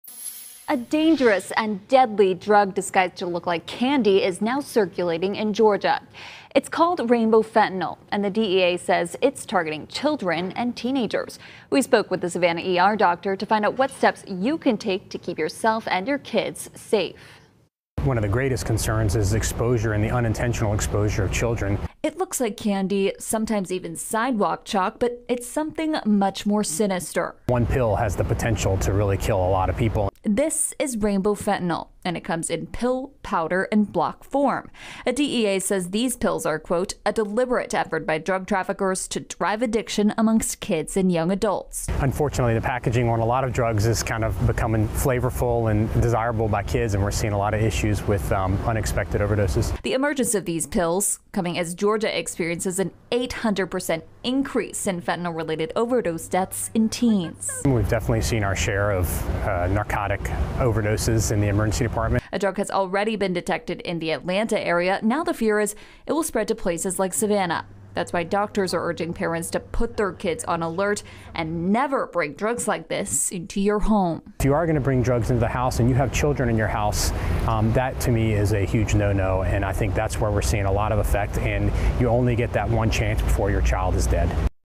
'Rainbow fentanyl' found in Georgia; Savannah doctor issues warning
A dangerous and deadly drug disguised to look like candy or sidewalk chalk is now circulating in Georgia.